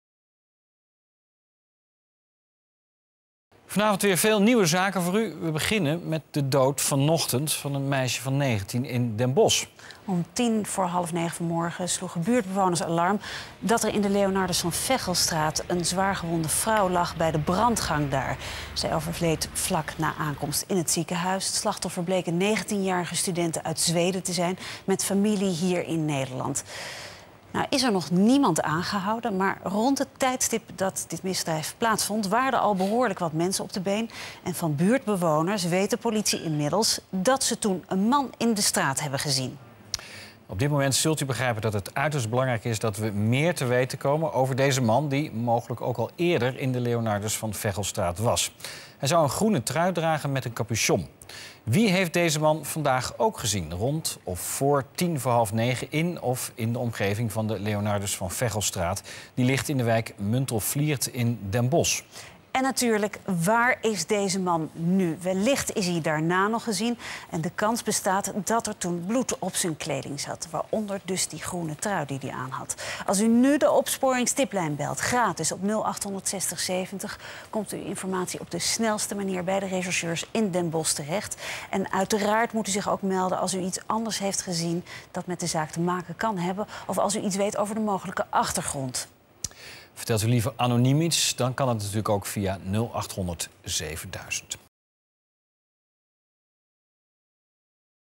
Den Bosch: Dood Zweedse studente (19) in brandgang van de Leonardus van Veghelstraat
Het jonge meisje overlijdt in het ziekenhuis aan haar verwondingen.